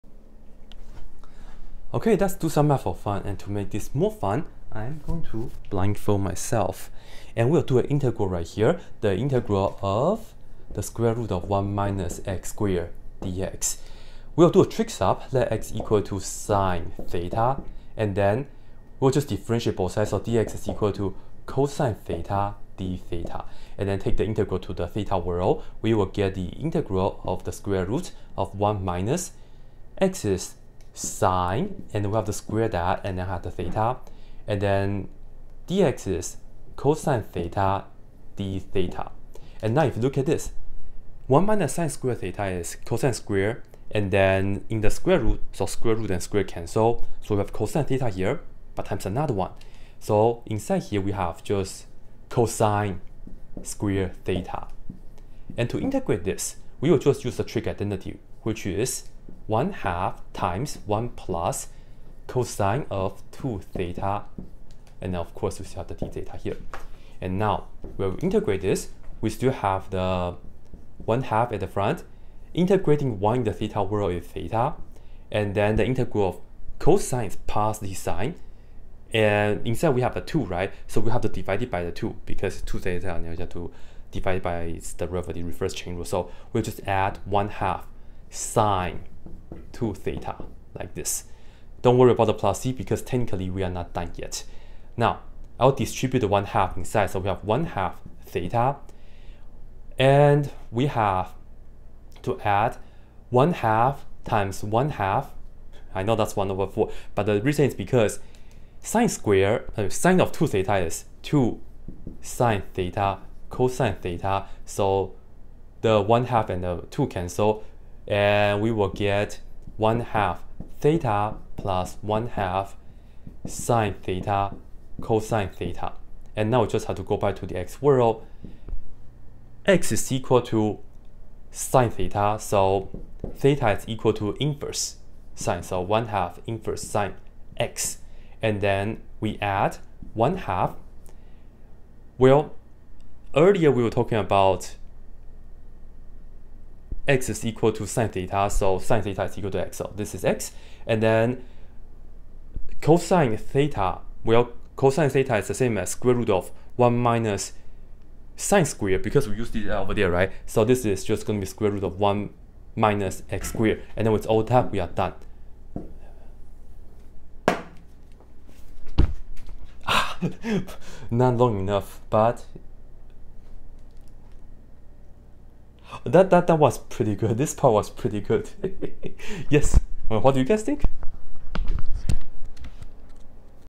when calculus teacher gets bored teaching normally
Teaching trig substitution whole blindfolded! I will perfect this stunt one day!